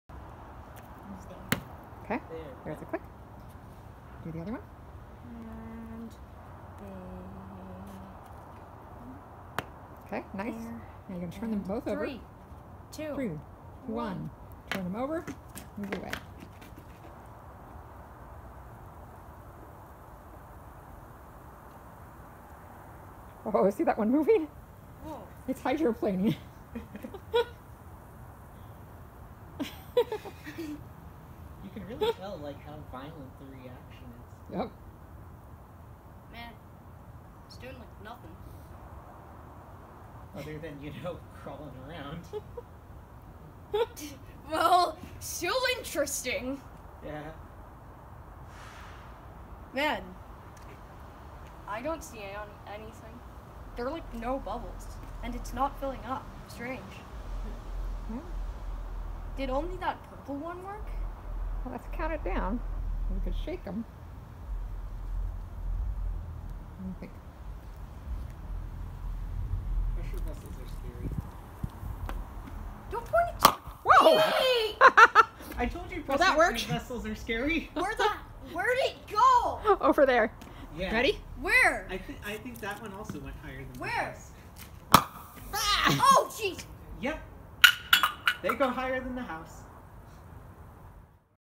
Film Canister rockets - baking soda and vinegar
Mix baking soda with just enough water to make a thick paste. Pack it into the inside of the lid of a film canister. Fill the canister halfway with vinegar. Snap the lid on. Turn it over and step back. Note: in the video, the set-up is done by 30 seconds... the rockets don't launch until 1:18 if you want to skip ahead to the excitement.